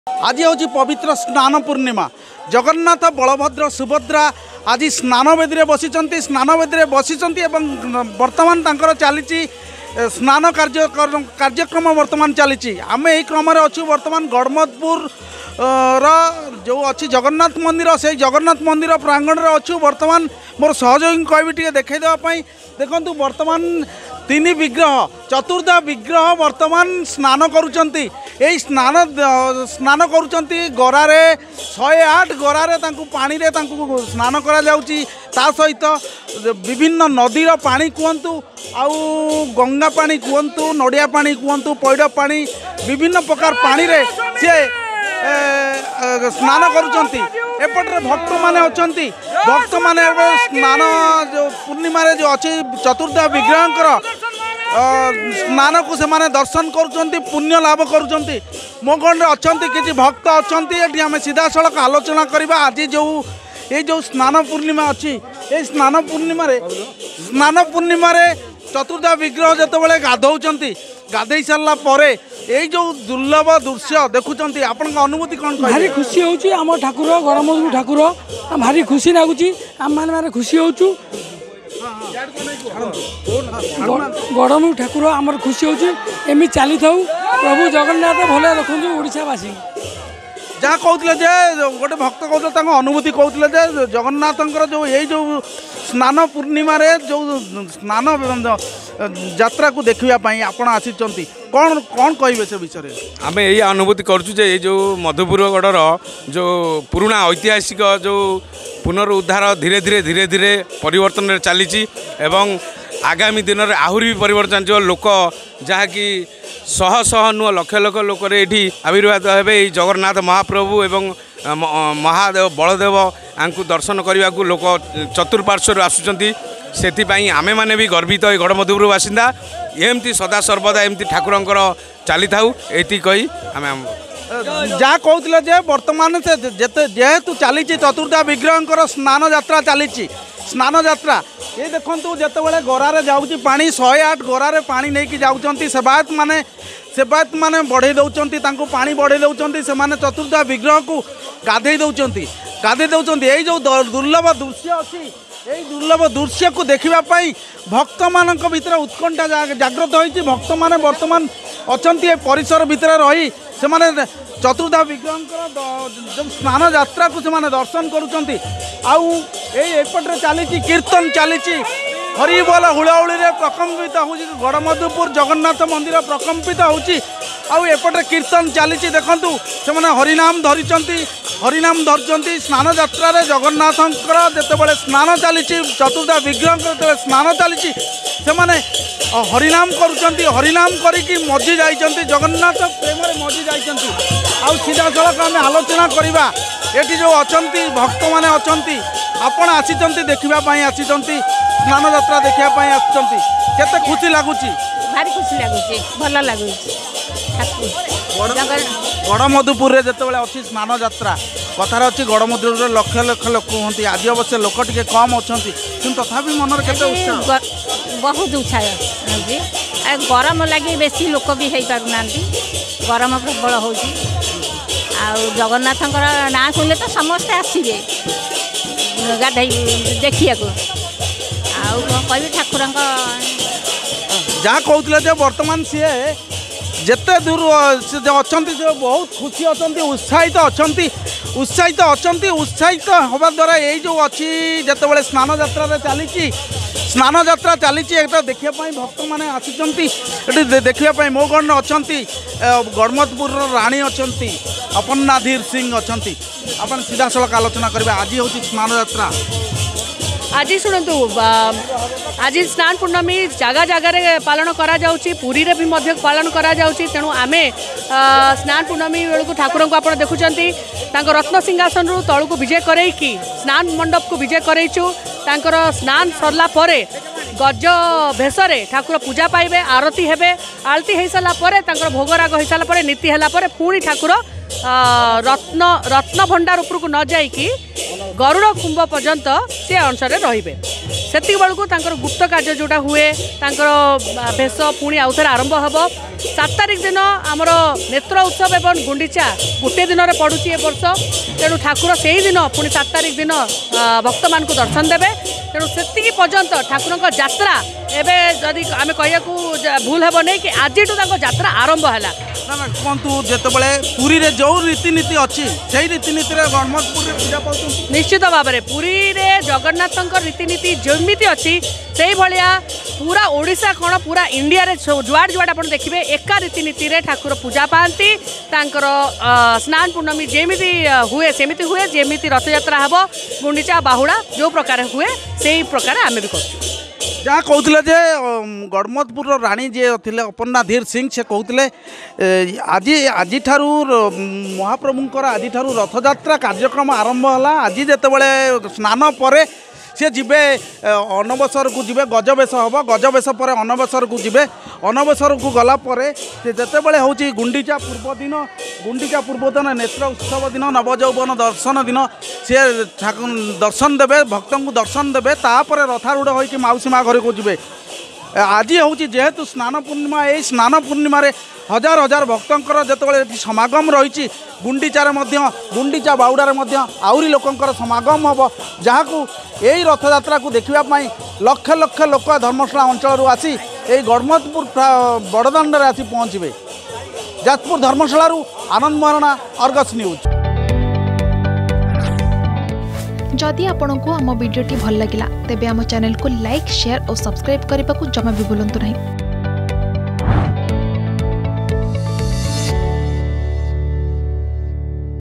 ଧର୍ମଶାଳା ଗଡ଼ମଧୁପୁର ଜଗନ୍ନାଥ ମନ୍ଦିରରେ ଚତୁର୍ଦ୍ଧା ମୂର୍ତ୍ତିଙ୍କ ସ୍ନାନ ଯାତ୍ରା || Snana Yatra 2024
ଧର୍ମଶାଳା ଗଡ଼ମଧୁପୁର ଜଗନ୍ନାଥ ମନ୍ଦିରରେ ଚତୁର୍ଦ୍ଧା ମୂର୍ତ୍ତିଙ୍କ ସ୍ନାନ ଯାତ୍ରା, ଏହାକୁ ଦେଖିବାକୁ ଭକ୍ତଙ୍କ ମଧ୍ୟରେ ଉତ୍ସାହ ।